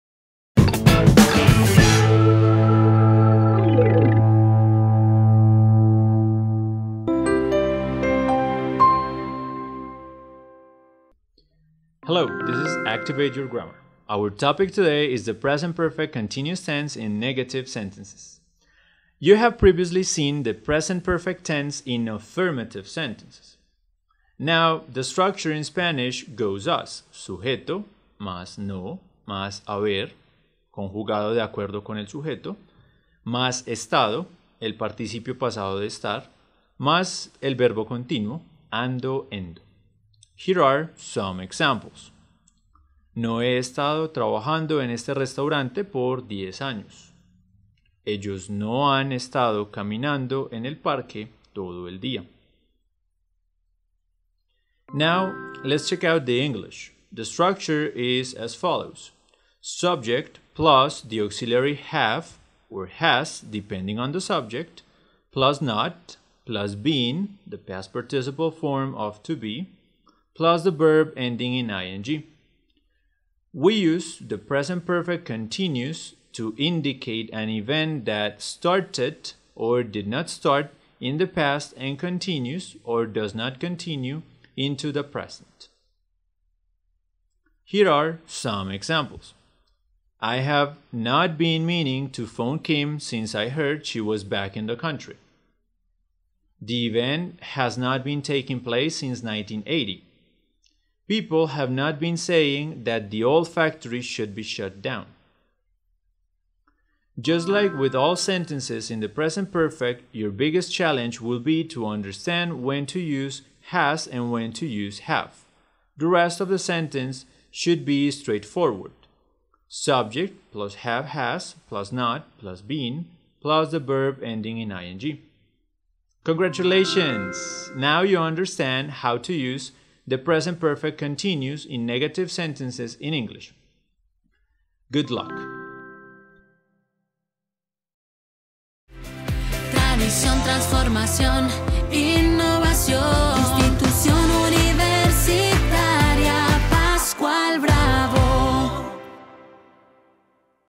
Activate Your Grammar: Present Perfect Continuous Negative
Our topic today is the Present Perfect Continuous Tense in Negative Sentence.
You have previously seen the Present Perfect Tense in Afirmative Sentences